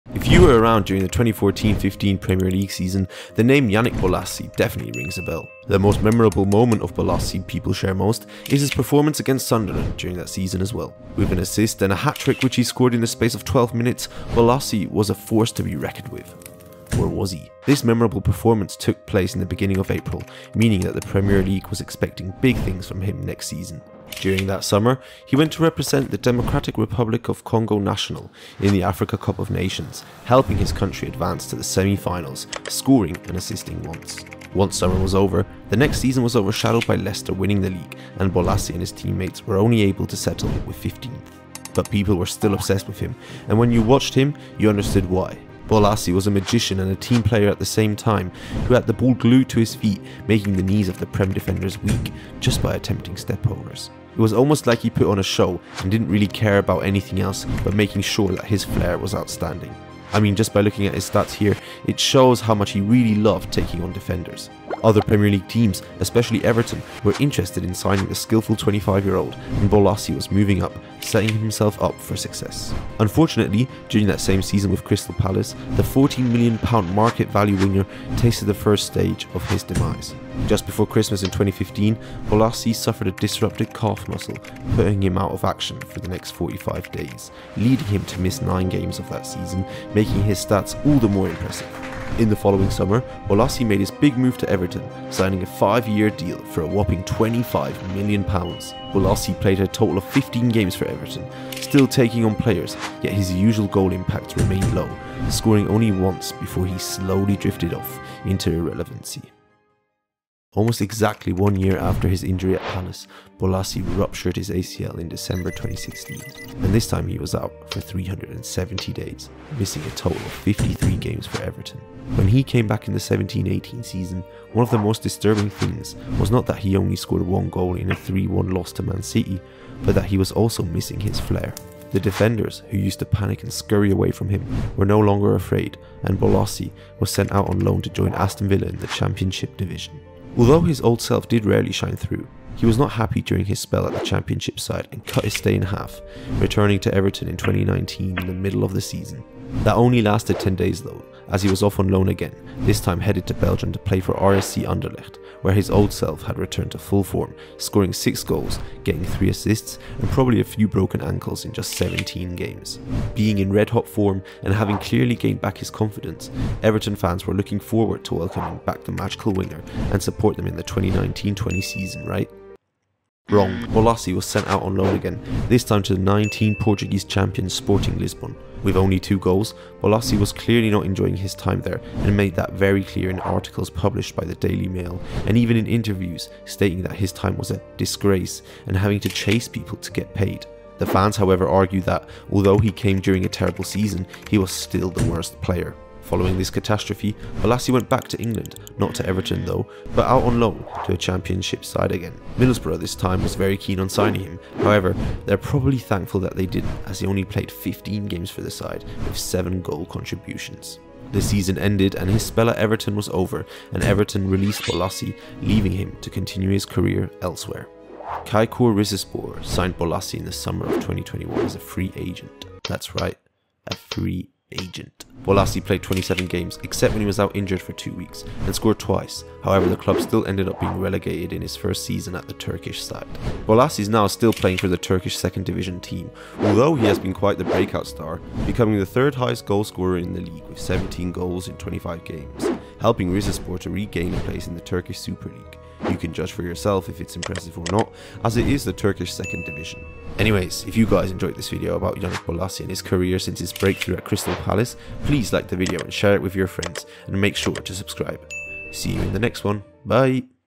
What happened to Yannick Bolasie?
Yannick Bolasie once dazzled the Premier League by showing off his skills and pace to everyone that he wasn't there to mess about! But what happened to him after he signed for Tottenham? How has he ended up in the Turkish Second Division?